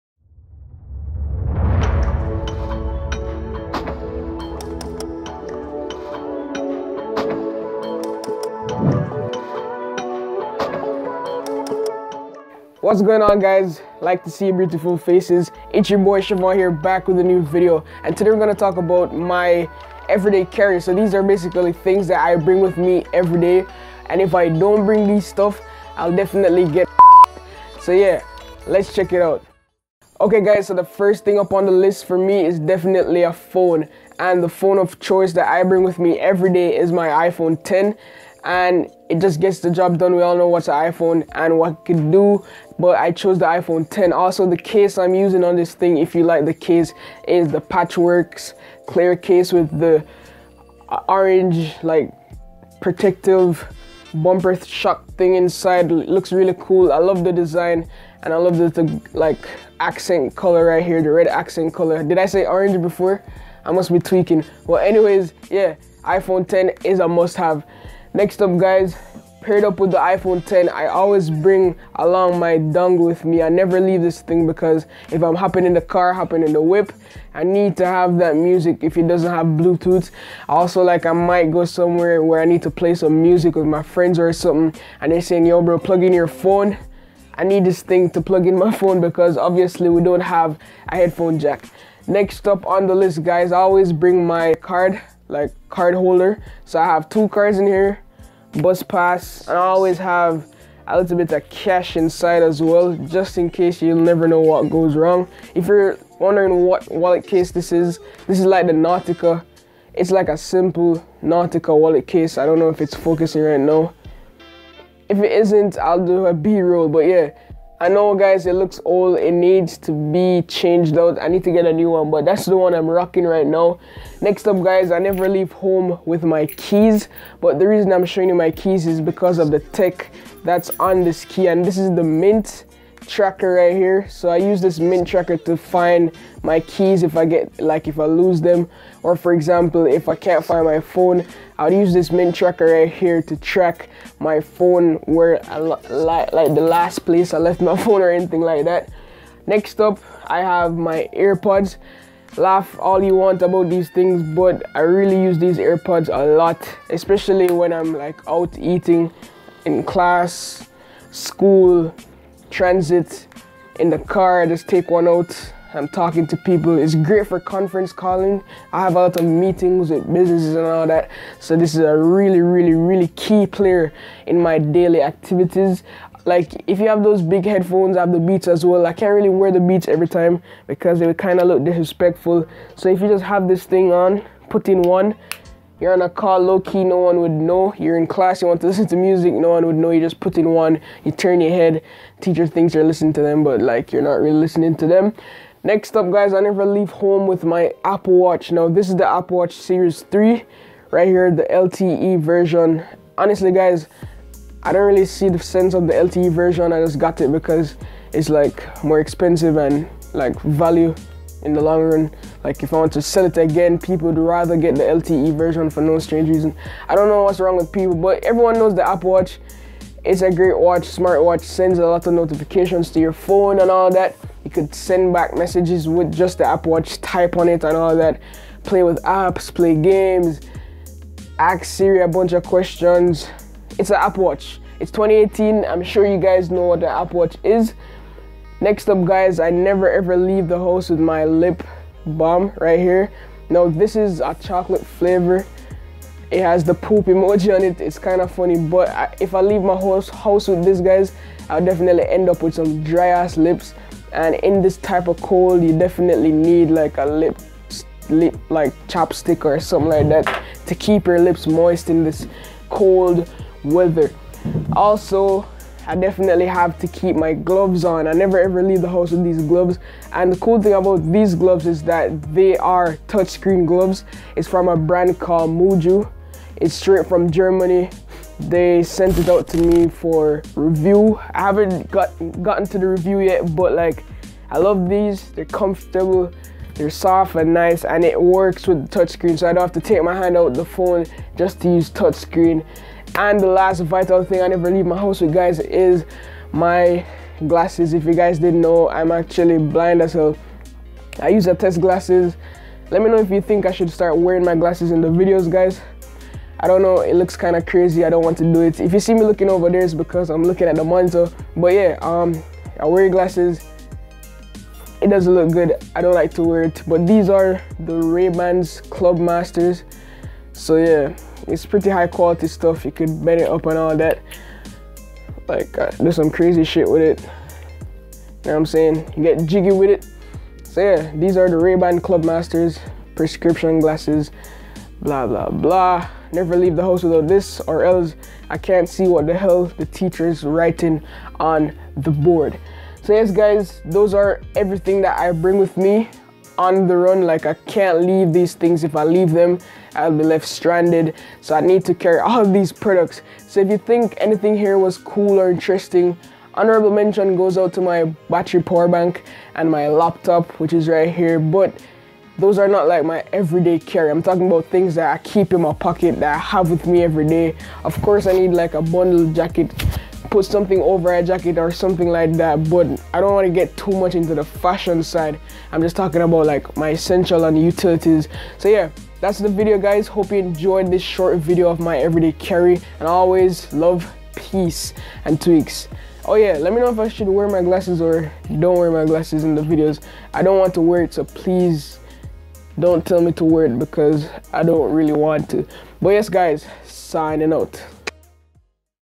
MY EVERYDAY CARRY 2018
Let me know your favorite tech accessories or your everyday carry down below in the comments.

Glass Screen Protector used
Wireless Charger I use

iPhone X

iPhone X Clear Case

iPhone Dongle Alternative

Nautica Wallet

Mint Tracker

Apple Airpods

Apple Watch

Urban Planet ChapStick: in store

Mujjo Touch Screen Gloves